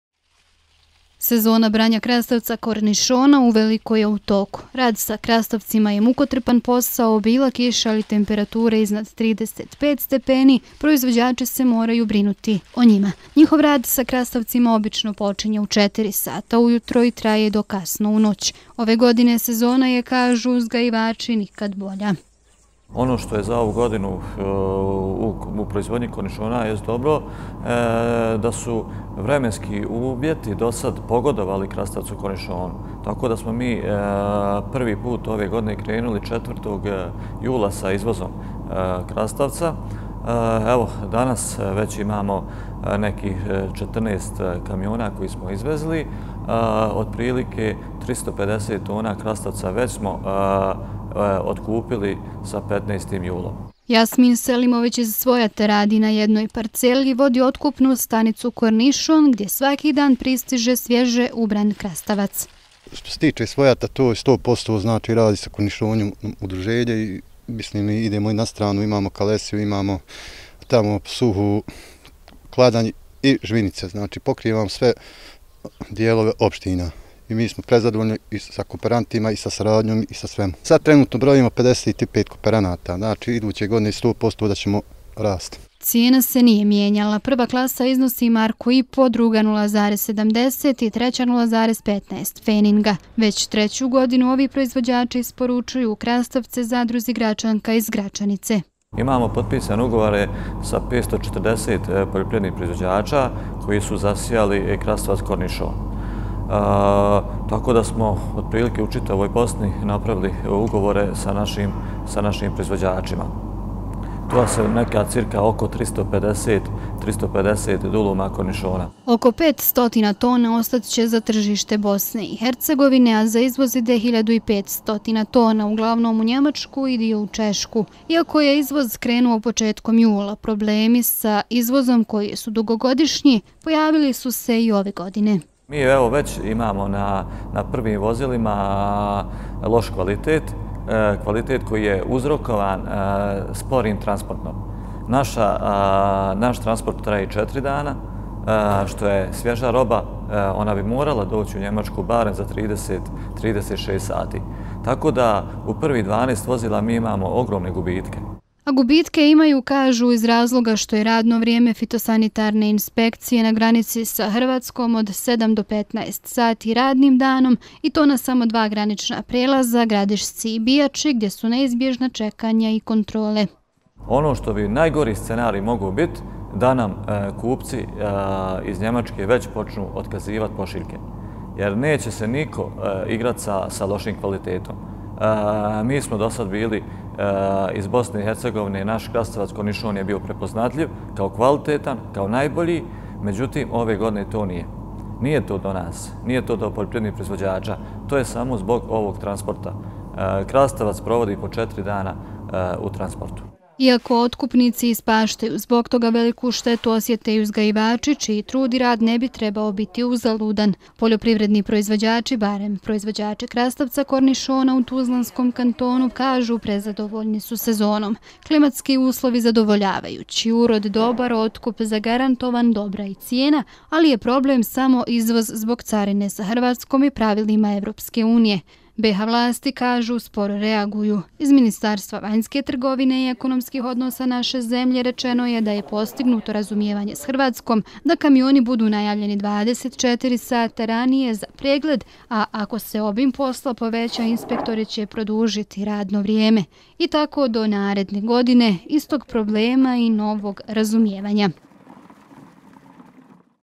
Vremenske prilike povoljne za uzgoj krastavca kornišona - 21.07.2021.
Dugo iščekivana kiša, koja je padala proteklih dana, te niže temperature nakon suše i izrazito toplog vremena, pogodno djeluju na usjeve i plodove, koji su za vrijeme vrućina znatno stradali, rekli su poljoprivrednici, ali i dalje gledaju u nebo i nadaju se novim padavinama koje bi dobro došle. Ipak smatraju da su klimatski uslovi zadovoljavajući, barem kada je u pitanju proizvodnja krastavca kornišona.